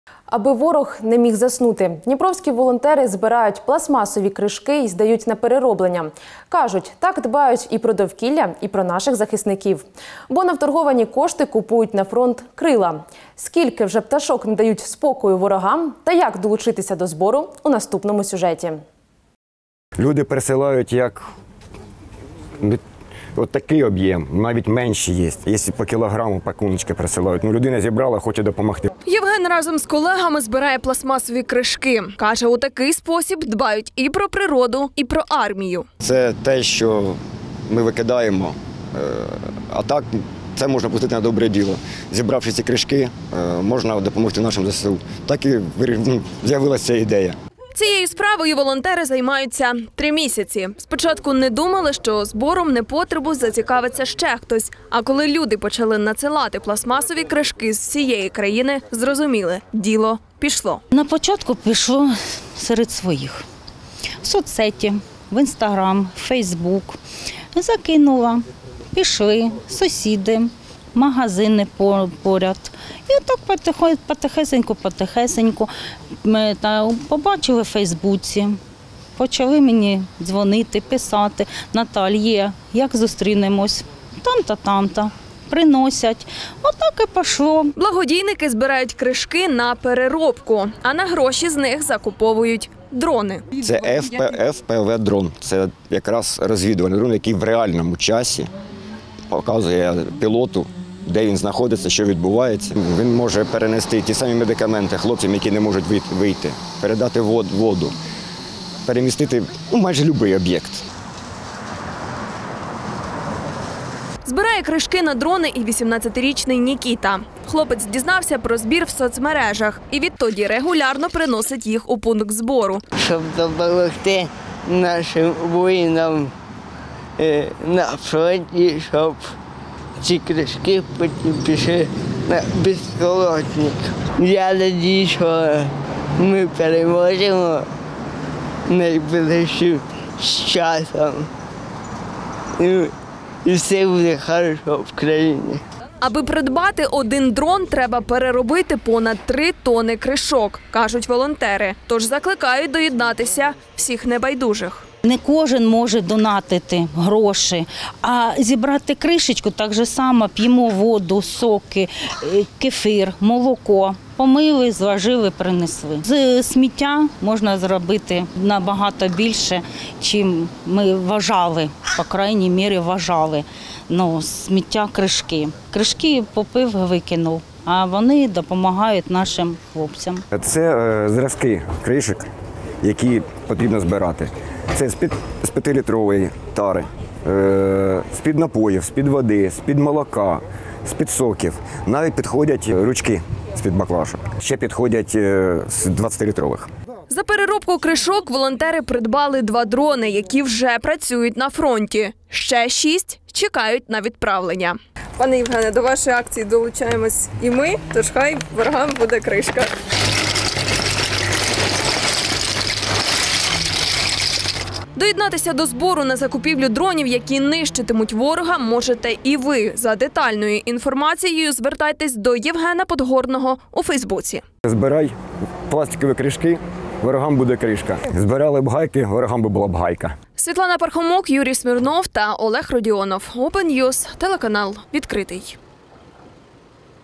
Дніпровські волонтери збирають пластмасові кришки на закупівлю дронів для ЗСУ
Аби ворог не міг заснути. Дніпровські волонтери збирають пластмасові кришки й здають на перероблення. Кажуть: так дбають і про довкілля, і про наших захисників. Бо на вторговані кошти купують на фронт - “крила”. Скільки вже пташок не дають спокою ворогам та як долучитися до збору - у наступному сюжеті.